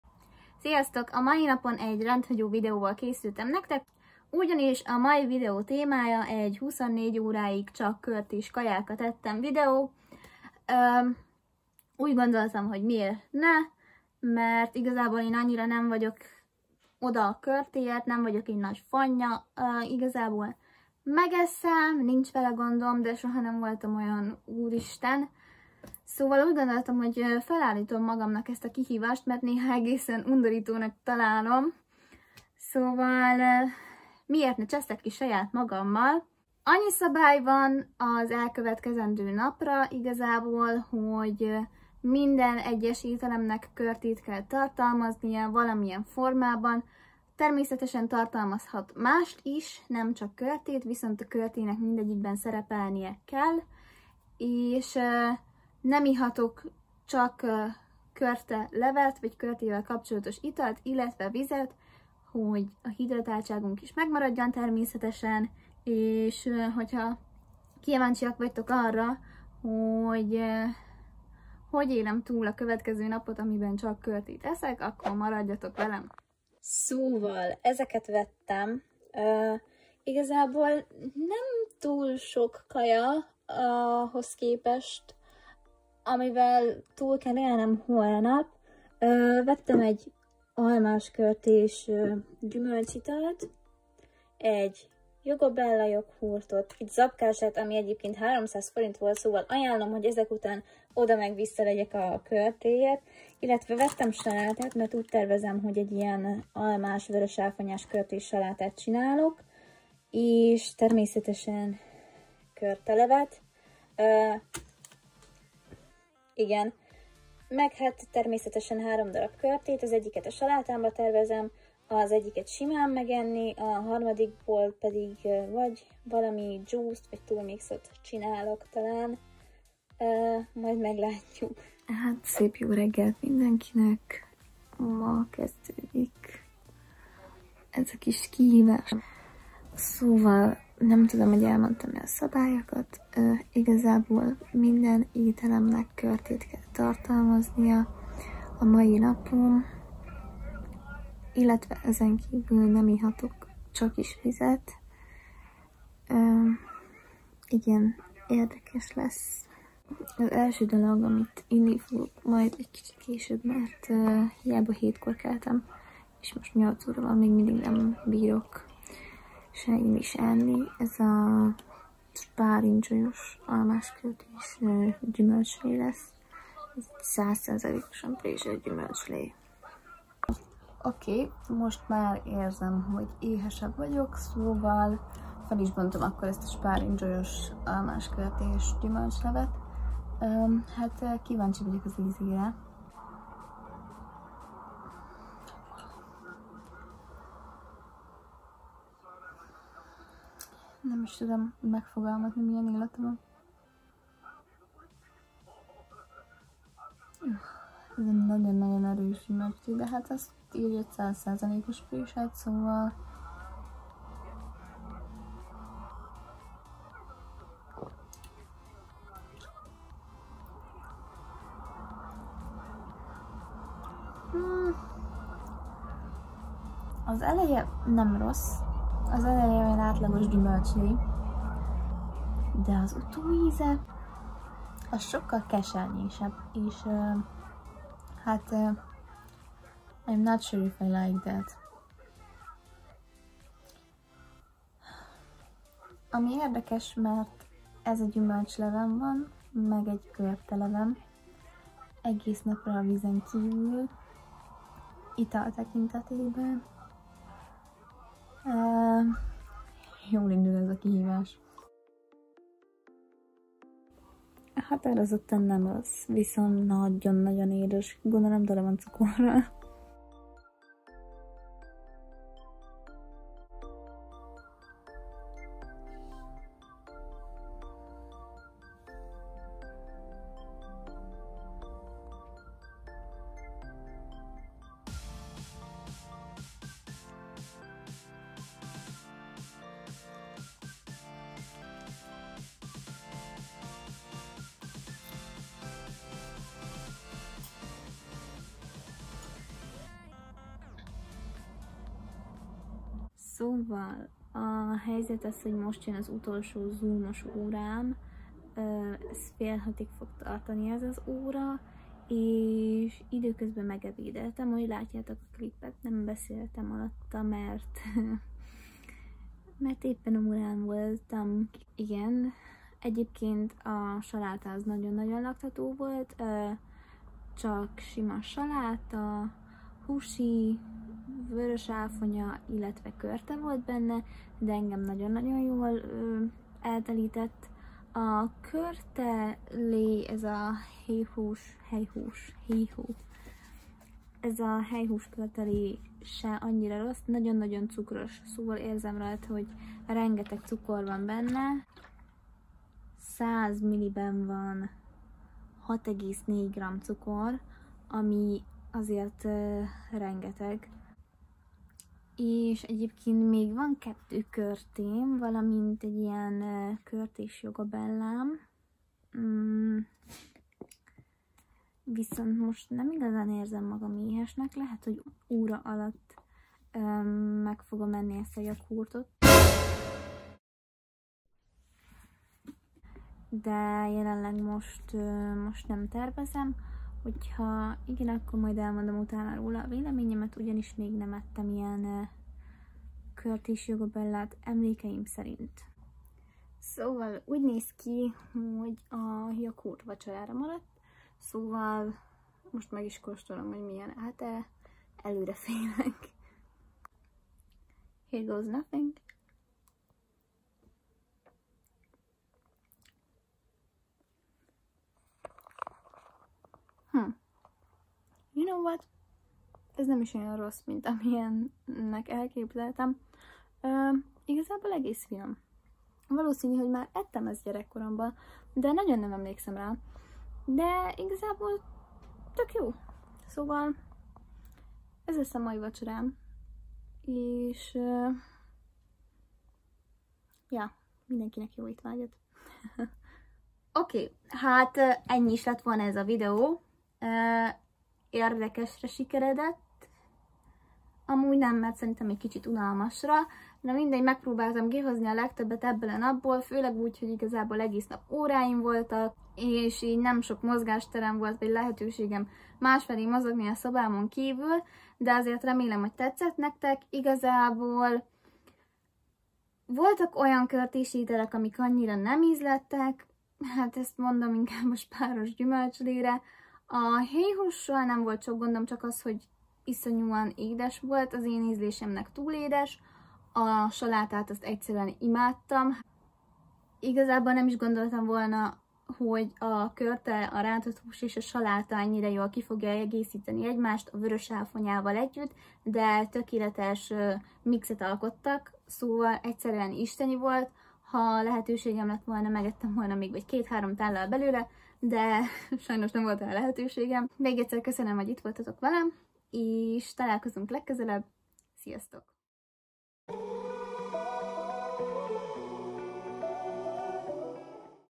EGY NAPIG CSAK KÖRTÉS KAJÁKAT ETTEM🍐| Kéri Dóri
A mai napon kipróbáltam néhány körtés ételt. Én magam annyira nem vagyok nagy rajongója, így kíváncsi voltam mennyire bírnám elviselni ha csak ilyen ételeket ennék 😝🍐

A videókat mind magamért, mind értetek csinálom :)
Ez az a hely, ahol megmutathatom önmagam, ahol végre én én lehetek és kiadhatom a véleményemet !